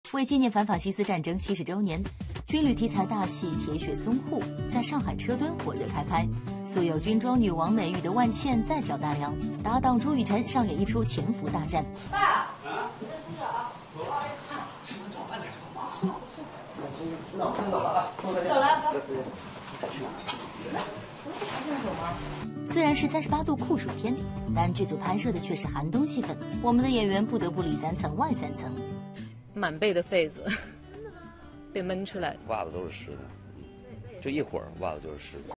《看看星闻》: 万茜朱雨辰车墩拍摄《铁血淞沪》Kankan News【SMG新闻超清版】
近日，由知名导演桑华执导，朱雨辰、万茜等人气实力演员主演的近代史诗巨制《铁血淞沪》爆出了首款主演定妆照。电视剧《铁血淞沪》以30年代的上海为背景，向观众真实呈现“一二八”淞沪会战期间，中国军队的英勇，以及风云际会、各种势力盘根错节的上海滩。义气男朱雨辰在剧中扮演一个为民族大义不惜一切的铁血战士。定妆照中，朱雨辰坚毅而深邃的眼神加之略显沧桑的妆容，将上海滩风云诡谲的气息挥洒的淋漓尽致。